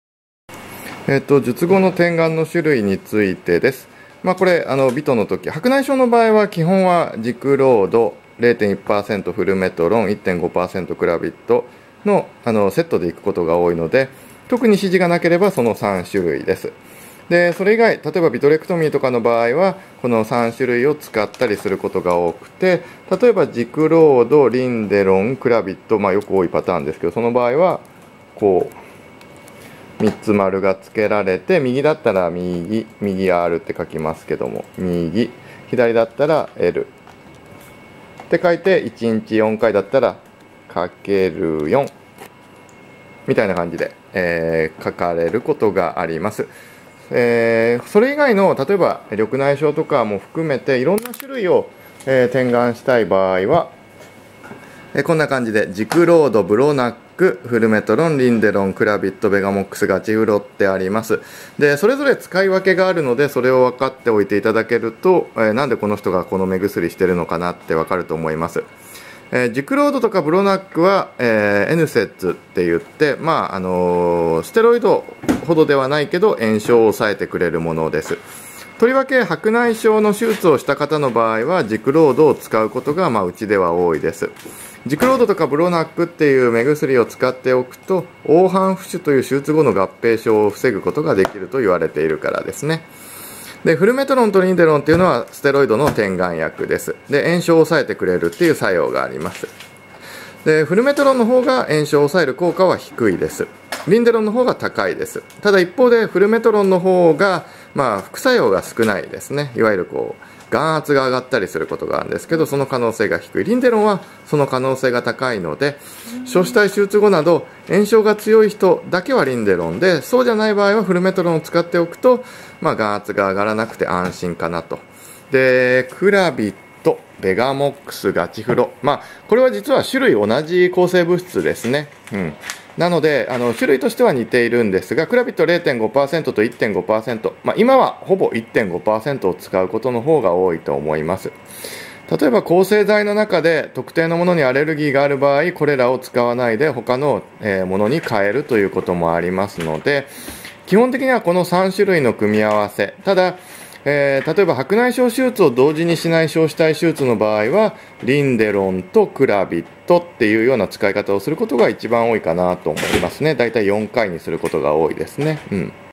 術後点眼指示「眼科職員教育」
二本松眼科の職員教育の一環として
術後点眼指示についてまとめたものです